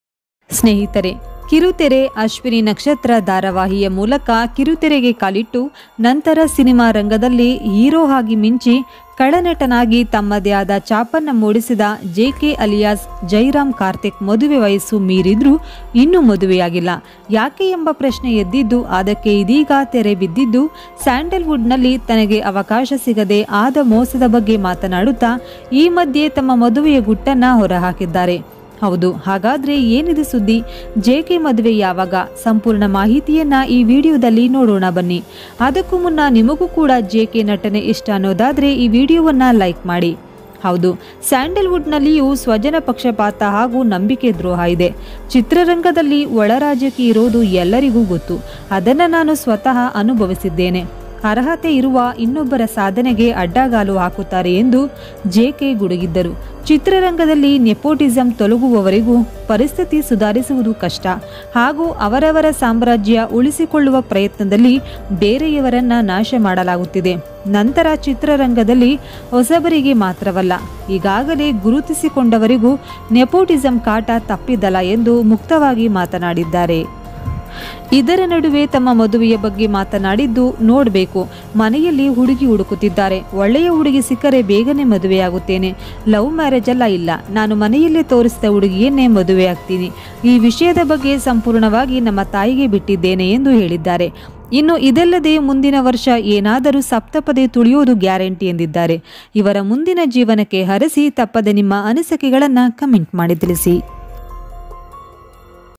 ದಿಡೀರ್ 42 ನೇ ವಯಸ್ಸಿಗೆ Jk ಮದುವೆ.! ಹುಡುಗಿ ಇವರೇ ನೋಡಿ.! ಶಾಕಿಂಗ್ | Kannada Actor Jk News
ದಿಡೀರ್ 42 ನೇ ವಯಸ್ಸಿಗೆ Jk ಮದುವೆ.! ಹುಡುಗಿ ಇವರೇ ನೋಡಿ.! ಅಶ್ವಿನಿ ಕಣ್ಣೀರು  News